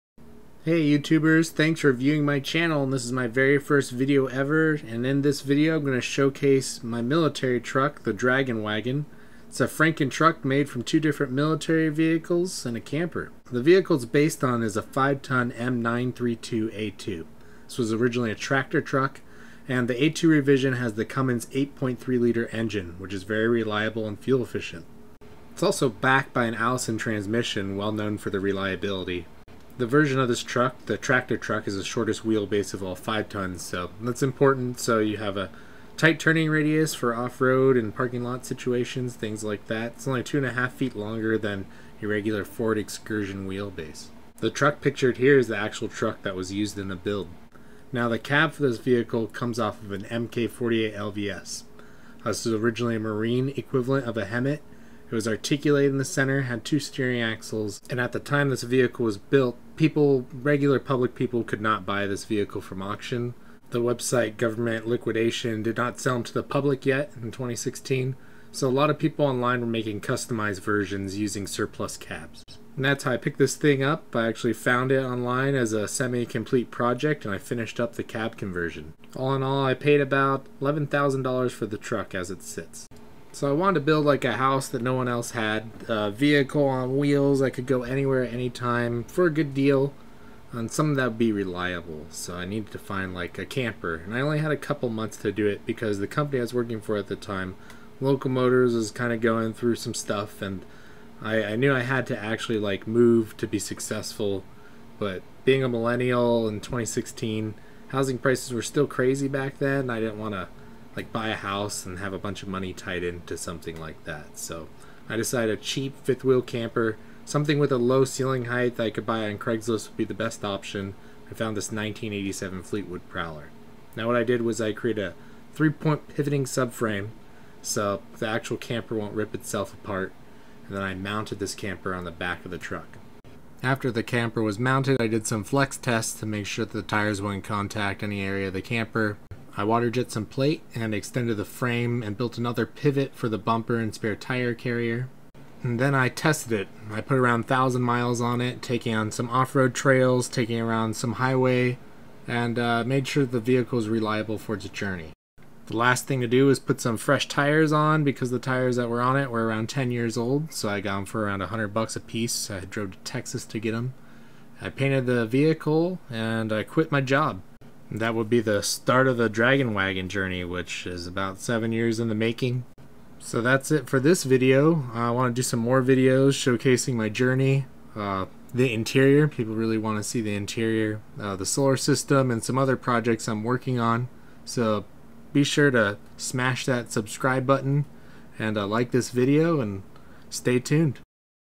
The DragonWagon, A DIY Frankenstein Military Truck Camper
The intro on how I made the Dragon Wagon. A completely DIY expedition truck build made from a 5ton Military Truck with the reliable Cummins 6CTA 8.3L engine, a HEMTT stylized cab swap and a 1987 Fleetwood Prowler.